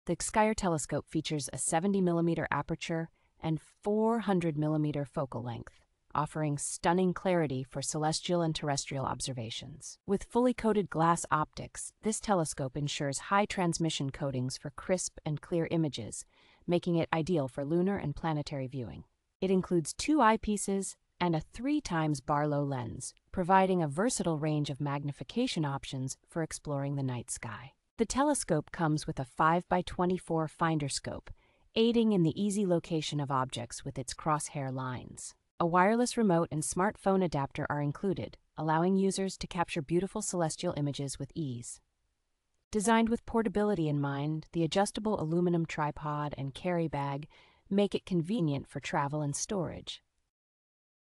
REVIEW (2024): Gskyer Telescope 70mm Aperture 400mm. ESSENTIAL details.
The Gskyer Telescope features a seventy millimeter aperture and four hundred millimeter focal length, offering stunning clarity for celestial and terrestrial observations.

With fully coated glass optics, this telescope ensures high transmission coatings for crisp and clear images, making it ideal for lunar and planetary viewing.

It includes two eyepieces and a three times Barlow lens, providing a versatile range of magnification options for exploring the night sky.

The telescope comes with a five by twenty-four finderscope, aiding in the easy location of objects with its cross-hair lines.

A wireless remote and smart phone adapter are included, allowing users to capture beautiful celestial images with ease.

Designed with portability in mind, the adjustable aluminum tripod and carry bag make it convenient for travel and storage.